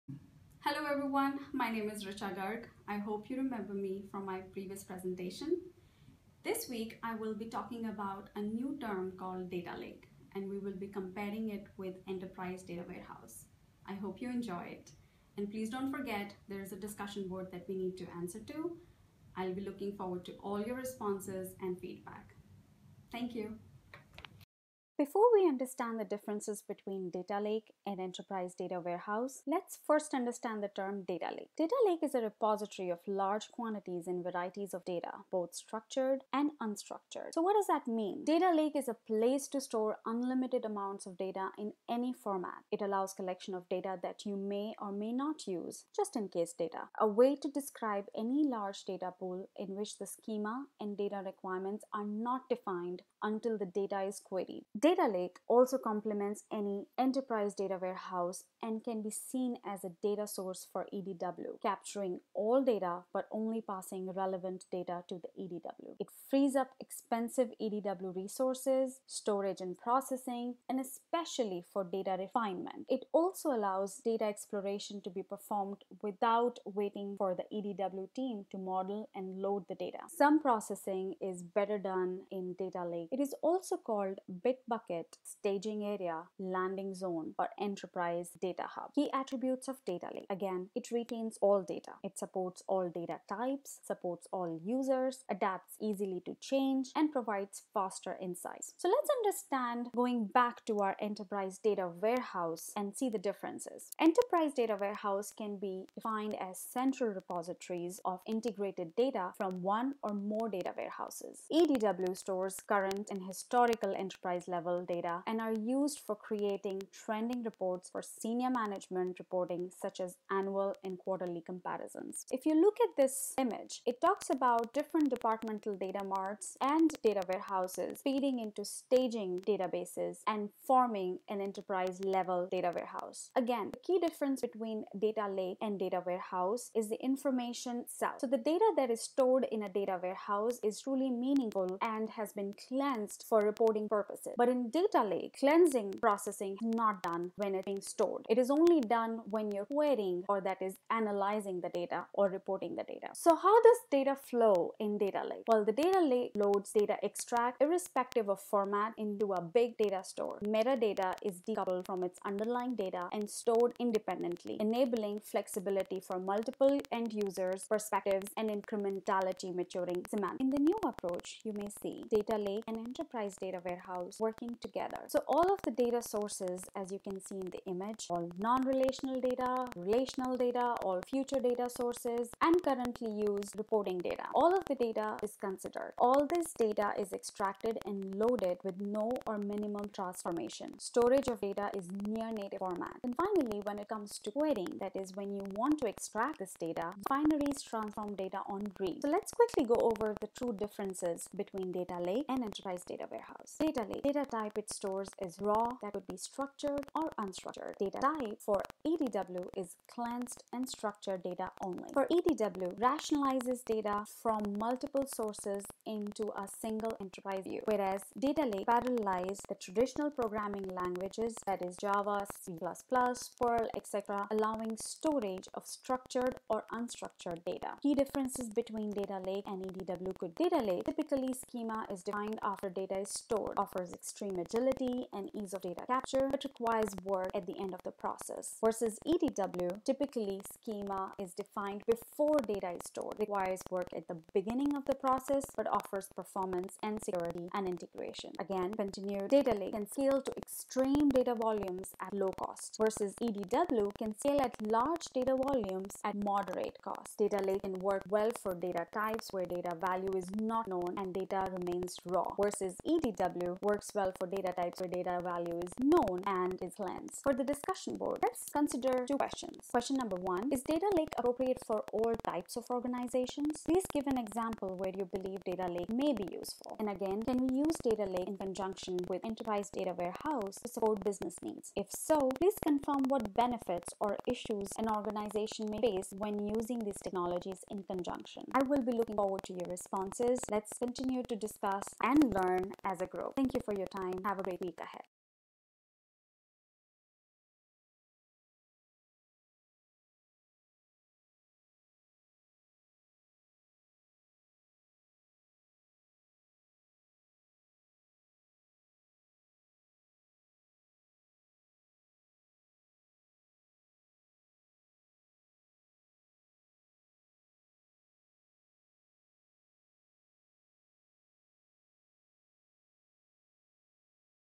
Data Lake vs. Enterprise Data Warehouse
ASU MSIM 2015 Class, CIS-591, presented by Richa Garg

References and additional reading:
Article: Super Bowl 43 & Business Intelligence – Prof. Alan Simon
Presentation: CIS 591 – Enterprise systems, Slide group 18 – Data warehousing architecture for Enterprise Systems
Presentation: CIS 591 – Enterprise systems, Slide group 19 – Data Governance and Leadership for Enterprise Systems